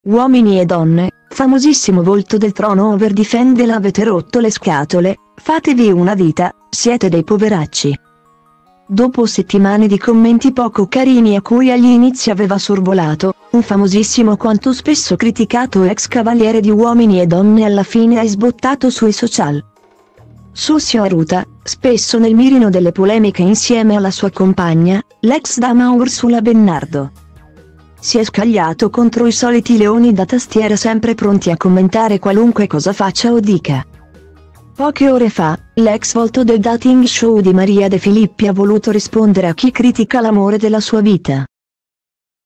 Uomini e Donne, famosissimo volto del Trono over difende la “Avete rotto le scatole, fatevi una vit
Uomini e Donne, famosissimo volto del Trono over difende la  “Avete rotto le scatole, fatevi una vita, siete dei poveracci!”
Dopo settimane di commenti poco carini a cui agli inizi aveva sorvolato, un famosissimo quanto spesso criticato ex cavaliere di Uomini e Donne alla fine è sbottato sui social. Sosssio Aruta, spesso nel mirino delle polemiche insieme alla sua compagna, l’ex dama Ursula Bennardo,si è scagliato contro i soliti leoni da tastiera sempre pronti a commentare qualunque cosa faccia o dica.